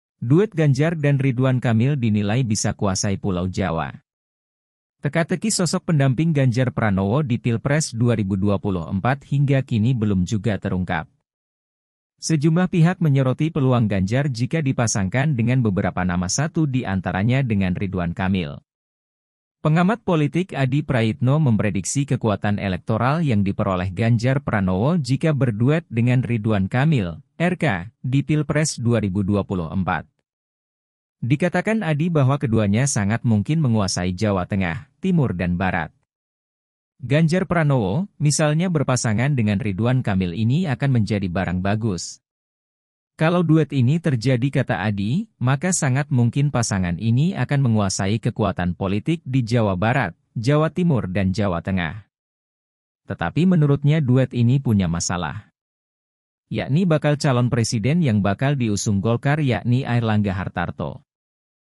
Duet Ganjar dan Ridwan Kamil Dinilai Bisa Kuasai Pulau Jawa, Begini Kata Pengamat
TRIBUNSOLO.COM - Teka-teki sosok pendamping Ganjar Pranowo di Pilpres 2024 hingga kini belum juga terungkap.

Sejumlah pihak menyoroti peluang Ganjar jika dipasangkan dengan beberapa nama satu di antaranya dengan Ridwan Kamil.

Pengamat politik Adi Prayitno memprediksi kekuatan elektoral yang diperoleh Ganjar Pranowo jika berduet dengan Ridwan Kamil (RK) di Pilpres 2024.

Dikatakan Adi bahwa keduanya sangat mungkin menguasai Jawa Tengah, Timur dan Barat.

"Kalau bicara tentang popularitas dan elektabilitas tentu Ridwan Kamil punya bekal itu. Bahkan dalam simulasi survei kalau Ganjar pranowo, misalnya berpasangan dengan Ridwan Kamil ini akan menjadi barang bagus," kata Adi kepada Tribunnews dikutip Rabu (5/7/2023).

Kalau duet ini terjadi kata Adi, maka sangat mungkin pasangan ini akan menguasai kekuatan politik di Jawa Barat, Jawa Timur dan Jawa Tengah

"Di mana tiga provinsi ini itu kekuatan suara pemilih sudah hampir mencapai 40 persen. Jadi Jawa itu adalah kunci," jelasnya.

Tetapi menurutnya duet ini punya masalah. Yakni bakal calon presiden yang bakal diusung Golkar yakni Airlangga Hartarto.

VP: TEGAR